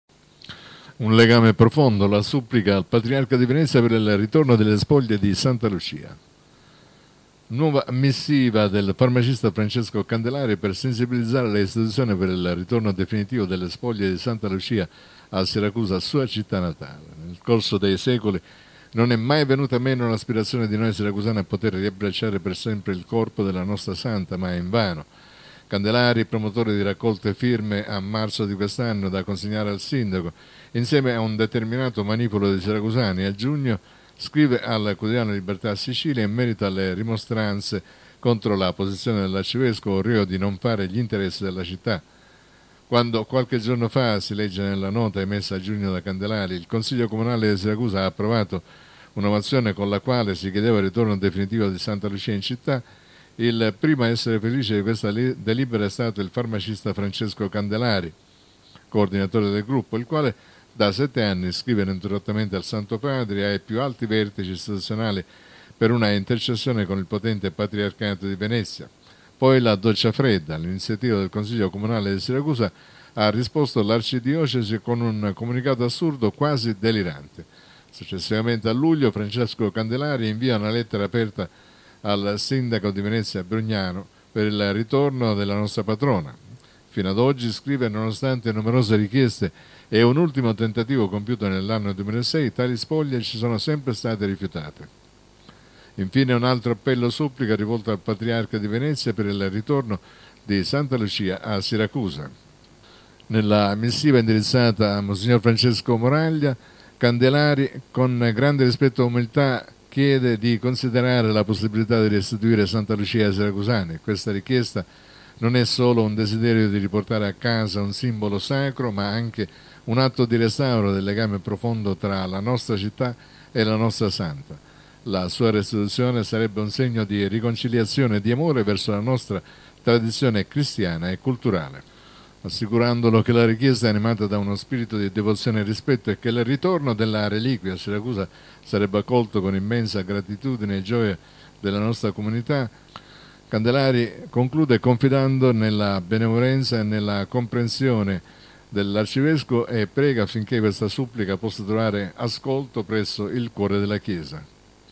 Un legame profondo, la supplica al Patriarca di Venezia per il ritorno delle spoglie di Santa Lucia
Nuova missiva del farmacista Francesco Candelari per sensibilizzare le istituzioni per il ritorno definitivo delle spoglie di Santa Lucia a Siracusa, sua città natale.

Nel corso dei secoli non è mai venuta meno l’aspirazione di noi siracusani a poter riabbracciare per sempre il corpo della nostra Santa, ma invano.

Candelari, promotore di raccolte firme a marzo di quest’anno da consegnare al Sindaco, insieme ad un determinato manipolo di siracusani, a giugno, scrivono al Quotidiano Libertà Sicilia in merito alle rimostranze contro la posizione dell’arcivescovo, reo di non fare gli interessi della città. «Quando qualche giorno fa – si legge nella nota emessa a giugno da Candelari – il consiglio comunale di Siracusa ha approvato una mozione con la quale si chiedeva il ritorno definitivo di Santa Lucia in città, il primo a essere felice di questa delibera è stato il farmacista Francesco Candelari (coordinatore del gruppo), il quale da sette anni scrive ininterrottamente al Santo Padre e ai più alti vertici istituzionali per un’intercessione con il potente Patriarcato di Venezia. Poi la doccia fredda: all’iniziativa del consiglio comunale di Siracusa ha risposto l’arcidiocesi con un comunicato assurdo, quasi delirante».

Successivamente, a luglio, Francesco Candelari invia una lettera aperta al Sindaco di Venezia Brugaro per il ritorno della nostra Patrona. «Sino a oggi – scrive – nonostante numerose richieste e un ultimo tentativo compiuto nell’anno 2006, tali spoglie ci sono sempre state rifiutate.

L’ennesima coraggiosa richiesta
Infine un altro appello-supplica rivolto al Patriarca di Venezia per il ritorno di Santa Lucia a Siracusa, che riceviamo e pubblichiamo di seguito integralmente.

“Eccellenza Mons. Francesco Moraglia,

Mi permetto di rivolgermi a Lei con umiltà e rispetto per esprimere un fervente desiderio e una supplica che nasce dal cuore di una comunità devota e devota a Santa Lucia. Sono Francesco Candelari, cittadino di Siracusa, e desidero presentare una richiesta che riflette il profondo legame spirituale e culturale della nostra città con la nostra amata Santa Lucia.

Siracusa, città natale di Santa Lucia, ha per secoli venerato la sua memoria con devozione e amore. La sua presenza spirituale continua a essere una fonte di ispirazione e speranza per tutti noi.

Con grande rispetto e umiltà, Le chiedo di considerare la possibilità di restituire Santa Lucia ai Siracusani. Questa richiesta non è solo un desiderio di riportare a casa un simbolo sacro, ma anche un atto di restauro del legame profondo tra la nostra città e la nostra Santa. La sua restituzione sarebbe un segno di riconciliazione e di amore verso la nostra tradizione cristiana e culturale.

Comprendo che tale decisione richiede una riflessione attenta e una valutazione delle implicazioni spirituali e storiche. Pertanto, sono pronto a collaborare con Lei e con le autorità ecclesiastiche di Venezia per facilitare qualsiasi processo necessario per questo fine.

Le assicuro che la richiesta è animata da uno spirito di devozione e rispetto, e che il ritorno della reliquia a Siracusa sarebbe accolto con immensa gratitudine e gioia dalla nostra comunità.

Confido nella Sua benevolenza e nella Sua comprensione e prego affinché questa supplica possa trovare ascolto presso il cuore della Chiesa.

Con rispetto e devozione,

Francesco Candelari, Cittadino di Siracusa”.